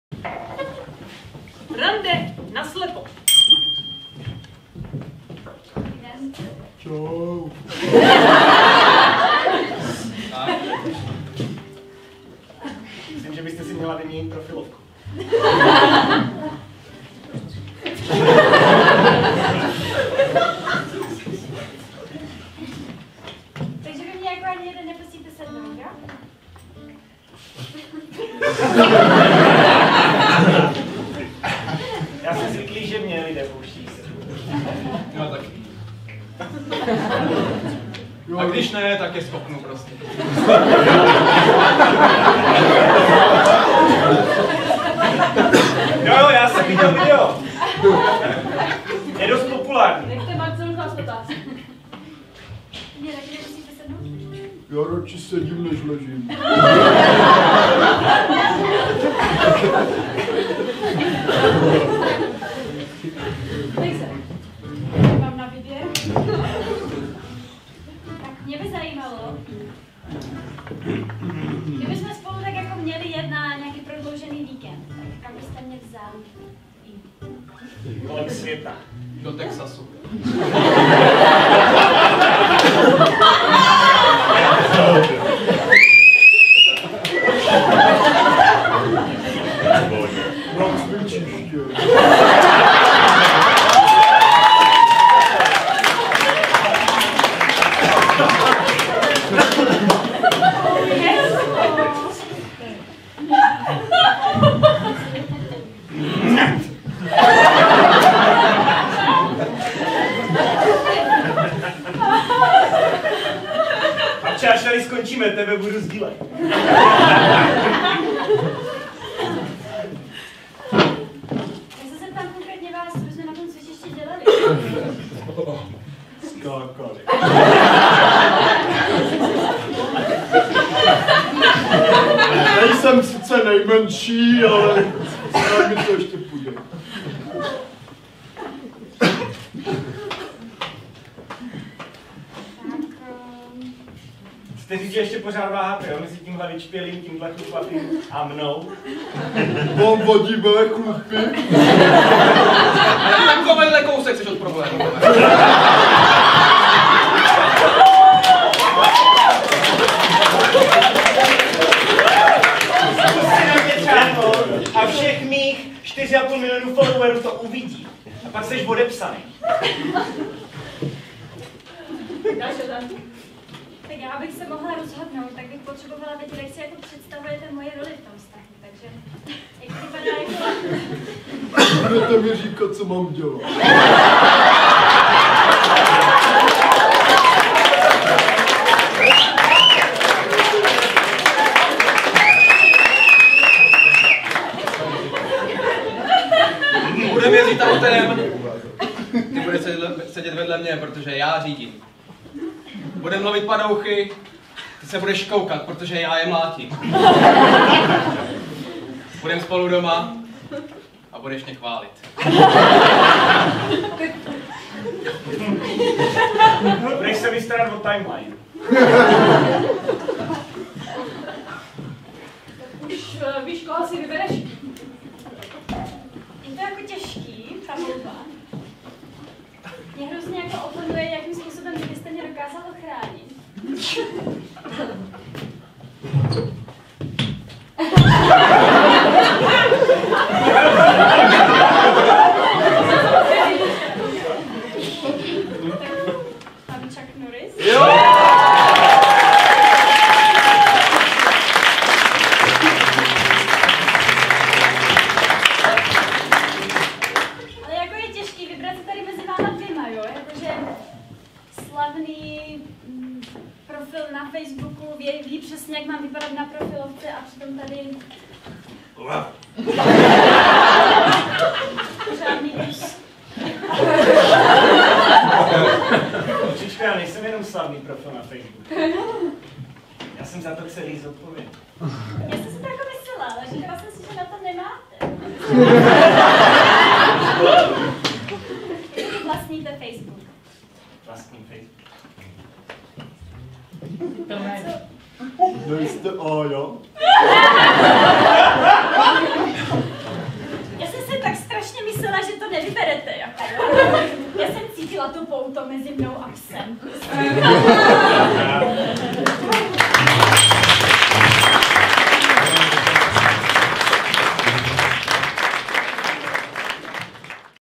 Rande na slepo
Pes, bijec, nebo technologický guru? Koho si Marcelka nakonec vybere? Podívejte se na záznam z našeho představení Ženatí versus Svobodní!